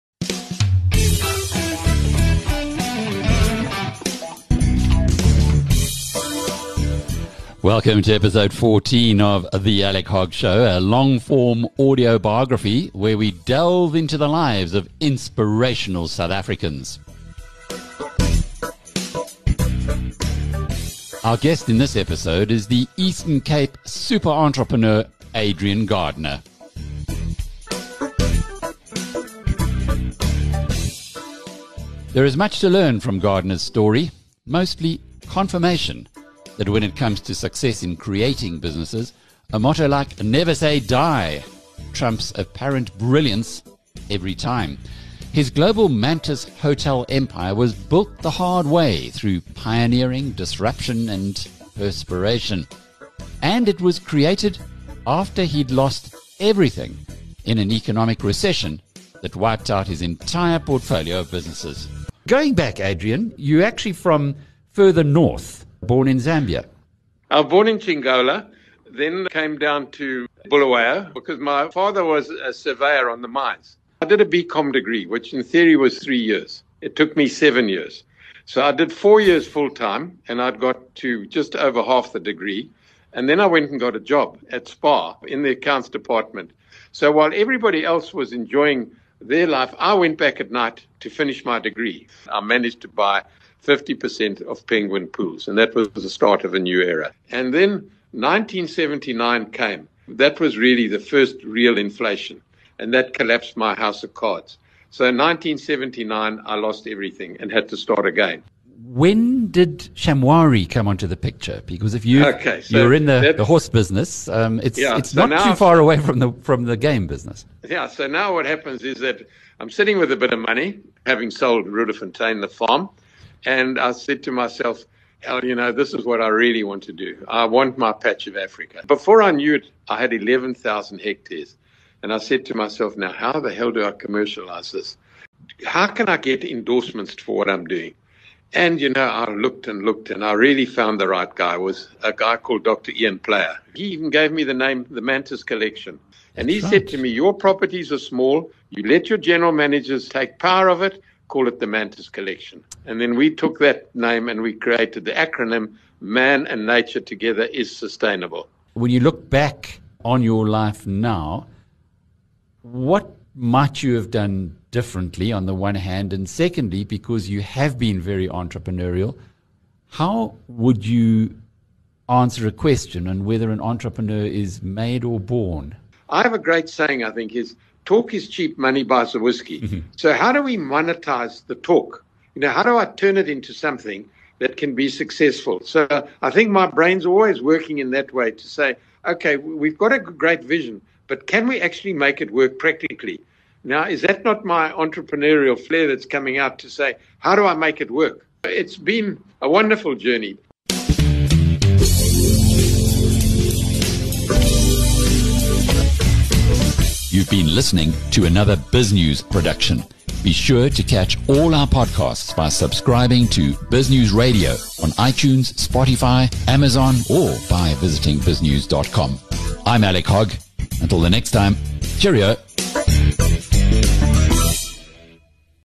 Comeback Kid Adrian Gardiner – from bankruptcy to luxury hotel mogul
The focus of this episode is one of South Africa’s most successful entrepreneurs, Port Elizabeth’s Adrian Gardiner. The inspirational story of this low profile family man exposes the ingredients needed by all successful entrepreneurs: curiosity, tenacity, humility, grit and, most of all, the self-belief to dust yourself off and start again. Gardiner lost everything in 1979 when an economic recession pushed his highly geared businesses into bankruptcy. But within a decade he was back on top of the local pond – and through his Mantis Group, making inroads on the continental and global stage. A fascinating story of a man who, at 77, despite being heavily invested in the Covid-19 affected hospitality sector, retains all the optimism of his youth